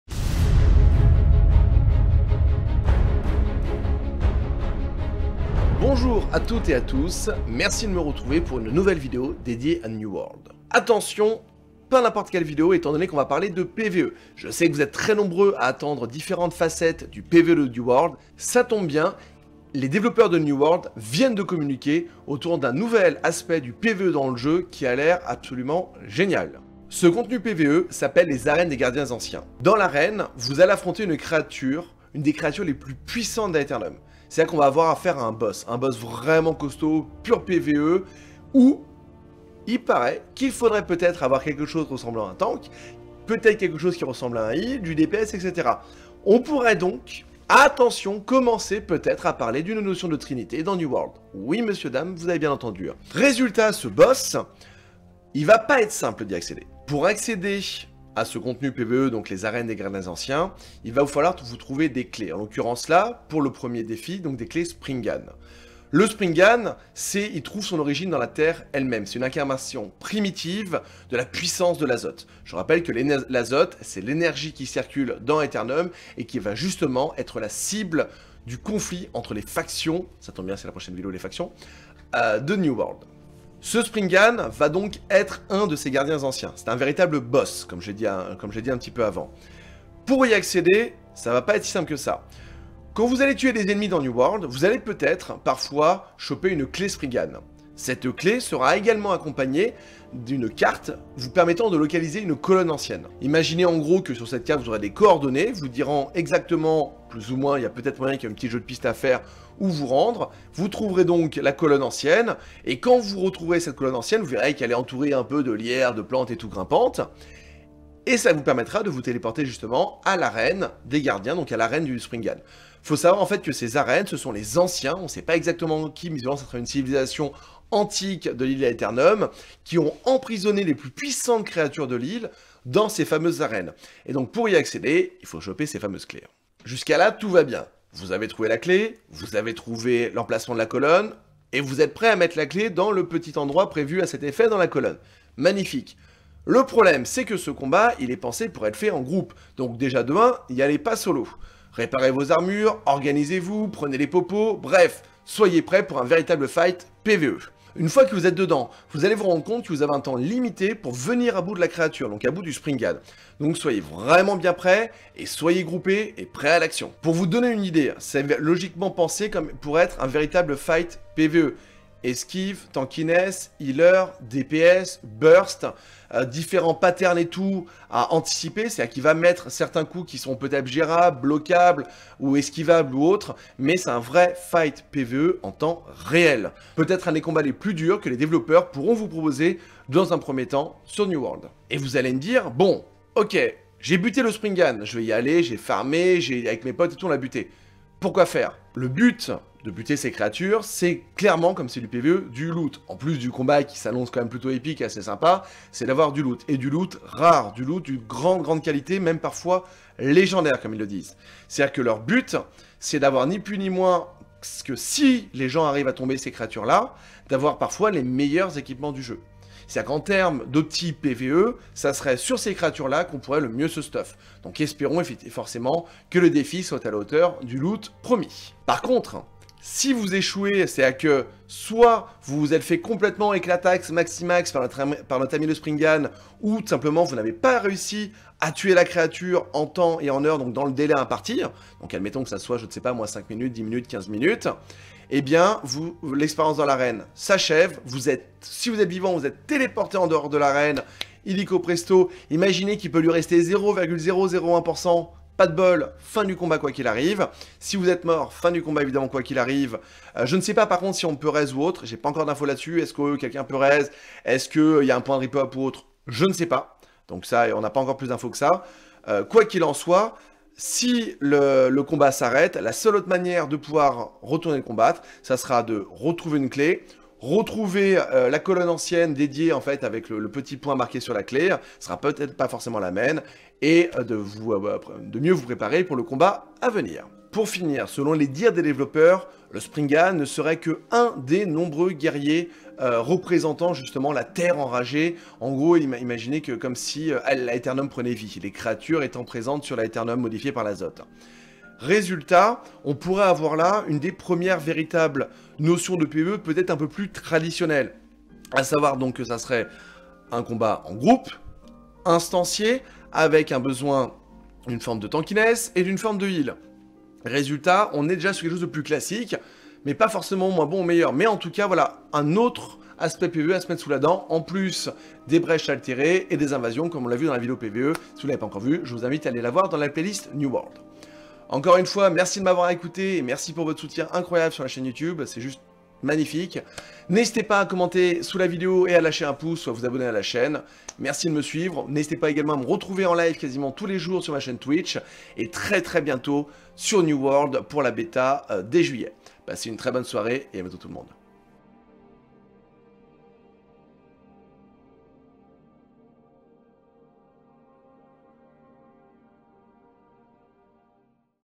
Du PVE Instancié dans New World !?
Les développeurs viennent d'annoncer un nouveau type de contenu pour New World : du PVE en instance, on analyse ça en détails dans cette vidéo !